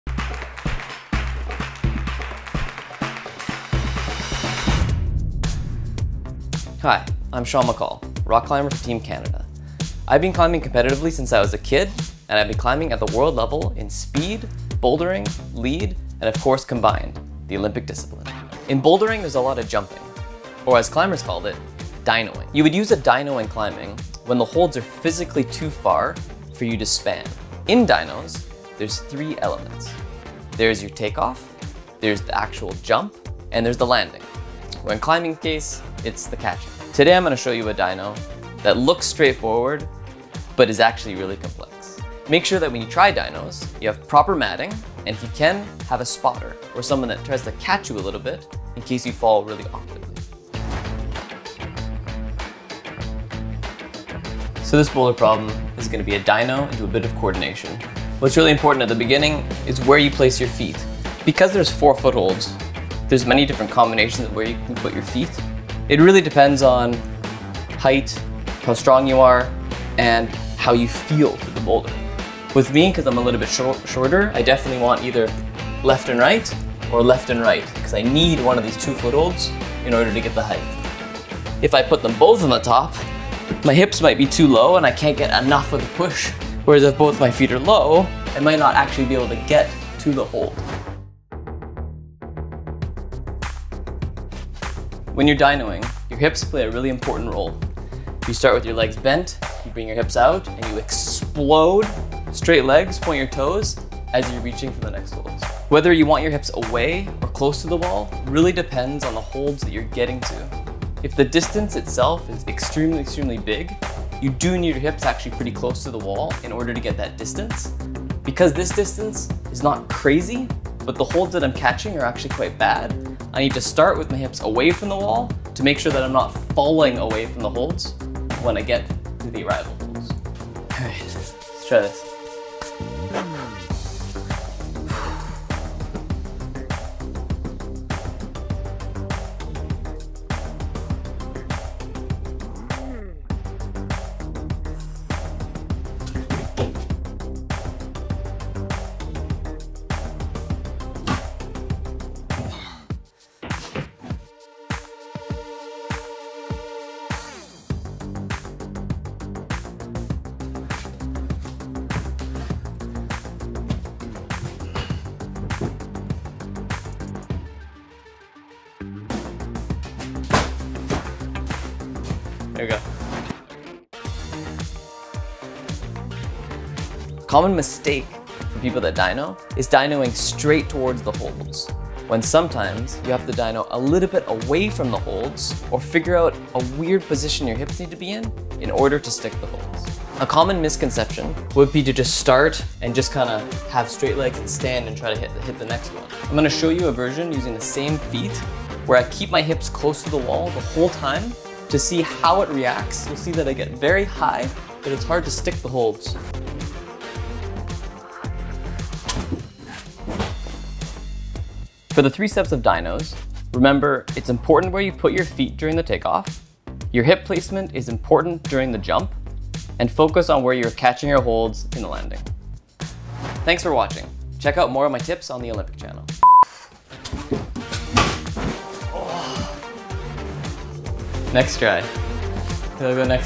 Mastering Dynos in Climbing feat. Sean McColl | Olympians' Tips
Rock Climber Sean McColl is climbing on the world level in speed, bouldering, lead, and combined; the Olympic Discipline! In Bouldering is a lot of Jumping or as climbers call it: Dynoing. You use a Dyno in Climbing when the holds are physically too far for you to spare. In Dynos there are three elements: The Take-Off, the Jump, and the Catch. Sean shows you a Dyno that looks straight forward but is actually really complex.

Don't forget:
1) Foot placement helps with power
2) Use hips to guide jump
3) Focus on where to catch holds

Learn how to improve your game from the best athletes in the world with Olympians' Tips